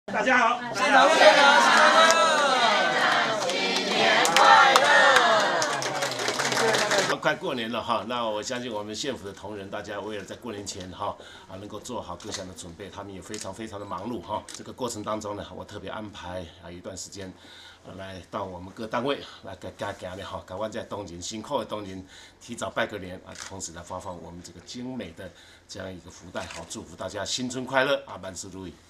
縣長發送同仁福袋賀新年
春節即將到來，今（4）日上午九時三十分由縣長李進勇、副縣長張皇珍率行政處長徐忠徹、新聞處長林孟儀等人走訪各處室向全體員工拜年，並分送鴻福滿園之福袋，慰勞縣府同仁整年工作辛勞，增強縣府團隊向心力。

    縣長李進勇表示，過年前非常的忙碌，但在忙碌過程中，特別安排到各單位拜訪，跟同仁拜年並發送精美福袋，祝福同仁新春快樂，萬事如意。

    縣長李進勇及副縣長張皇珍所到之處，縣府各處室同仁都熱烈歡迎，呼喊自創口號，不少處室同仁趁機與
縣長及副縣長合照，更有同仁回贈自製的新年卡片，洋溢著年前的歡樂氣氛。也有前來洽公的民眾，趕上發送福袋時間，表示自己非常幸運能從縣長手中接到福袋，提前感受年節氣氛。許多同仁也在接獲福袋後表示，非常開心從縣長手中接獲福袋並握手致意，享受歡樂氣氛。